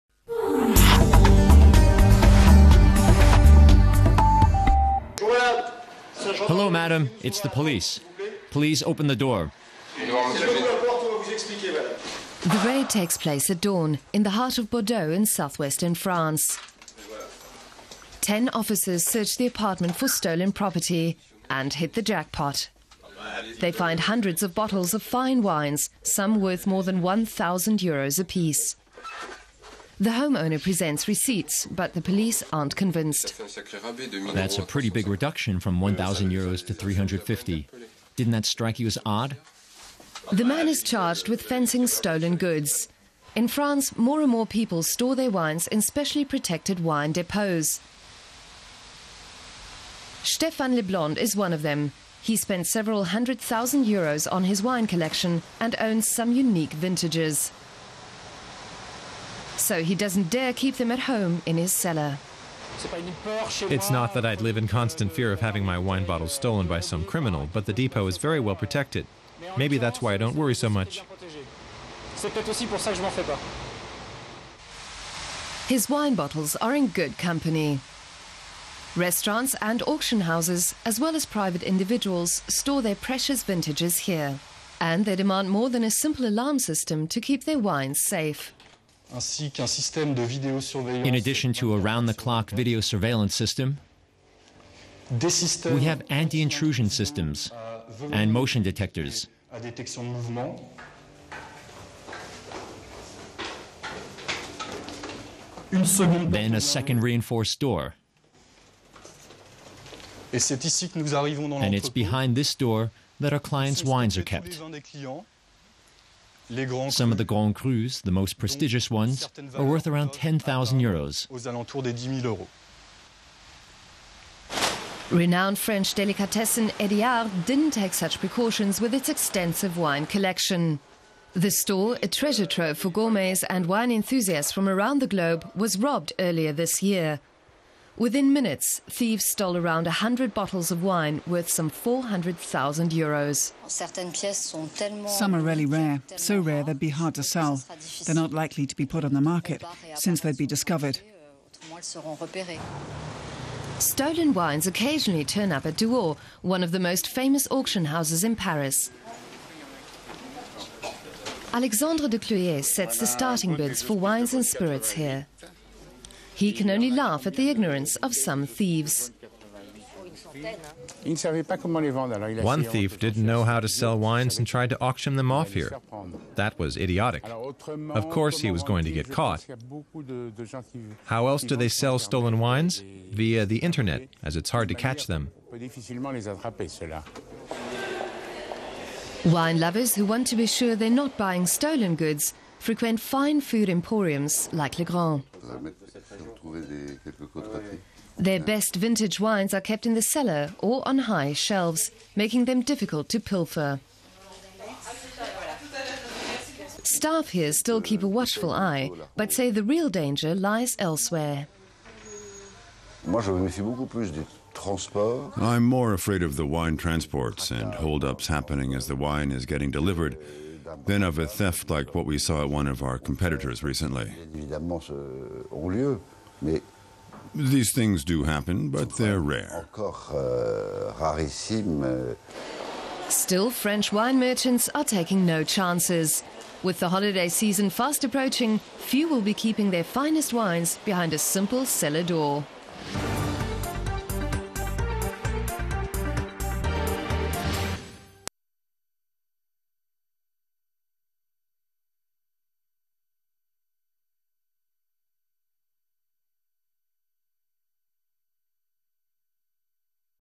France: Wine for a Steal | European Journal
Any time a classy wine is offered online below market price,be suspicious: the wines are often stolen. In France,owners of many a chateau are complaining about wine thievery. More and more often the gendarmerie are called to investigate when wine cellars suddenly go empty. Even big-name shops like Hédiard have been hit,just recently,someone tried to get their hands on some costly Grand Crus. More and more wine shops are turning to elaborate security procedures such surveillance cameras and heavy security doors.